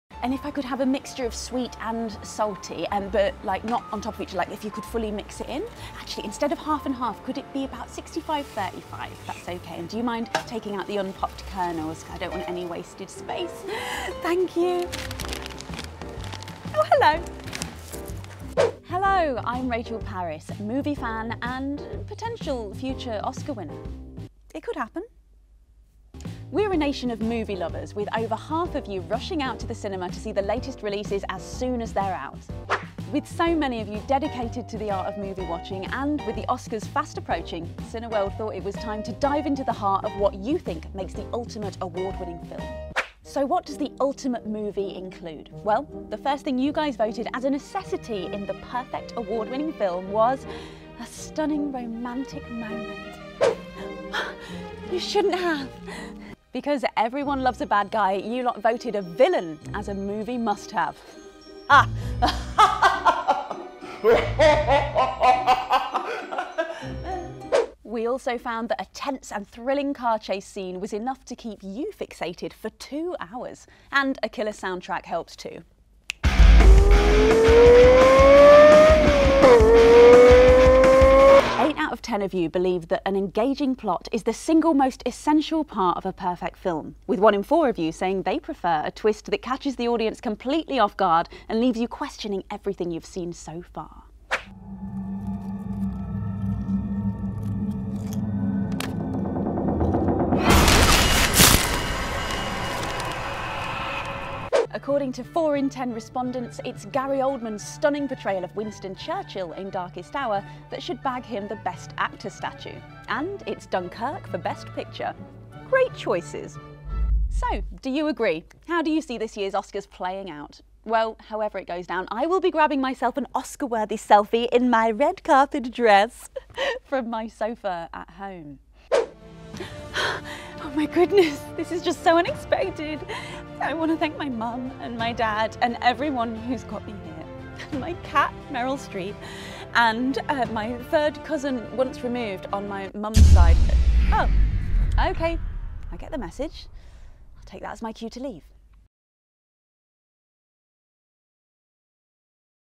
Cineworld Presents The Perfect Awards Film - with Rachel Parris
We asked over 2,000 of our Unlimited members what makes the perfect award-winning film. The lovely Rachel Parris is here to tell you the results - plus your guesses for who will win the Best Picture Oscar!

What's Cineworld Unlimited? Watch unlimited movies, get discount on cinema snacks, advance screenings & more, all from just £17.90 a month!

Already a member? Share your CineworldUnlimited story with us!